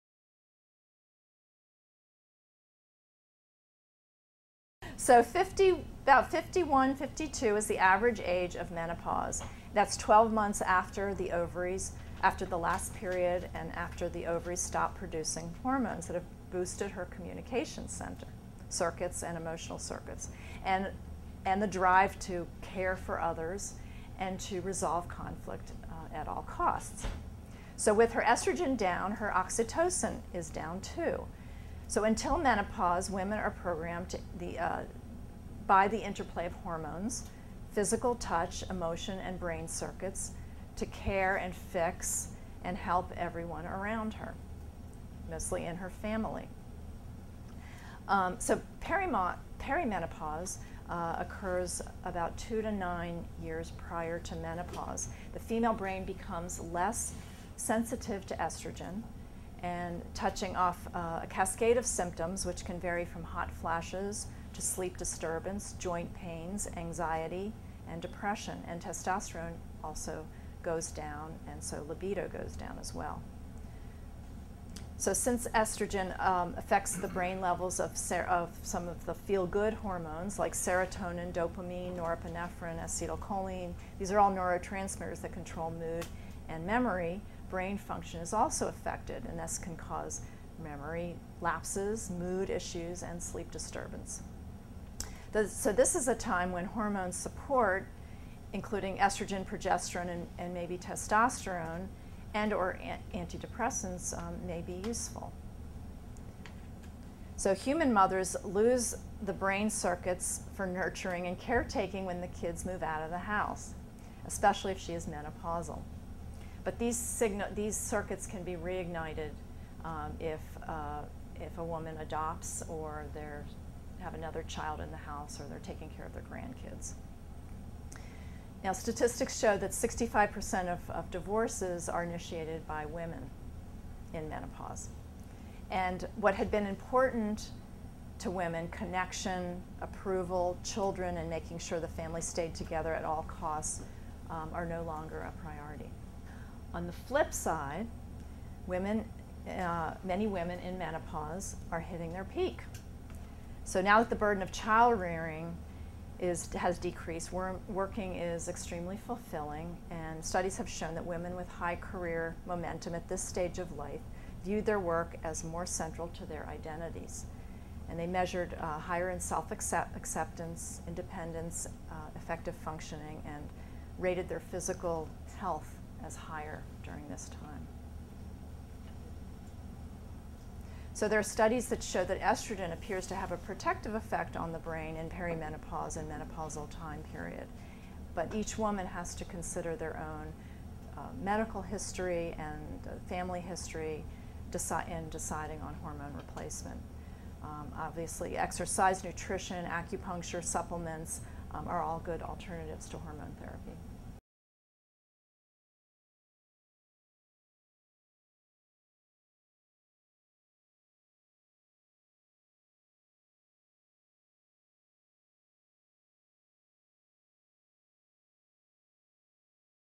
The Female Brain Video Series: Menopause (Part 6 of 6)
Dr. Lisa Lilienfield explains how, with the onset of perimenopause, a woman's estrogen and oxytocin levels begin to drop. By menopause, these hormonal changes can affect her brain function, emotions, and physical health (e.g., more memory lapses, increased irritability and joint pain). Some women elect hormone therapy. Others may seek alternative treatments, including physical exercise, acupuncture, meditation, and the use of nutritional supplements. Dr. Lisa's presentation is based on the book "The Female Brain" by Dr. Louann Brizendine.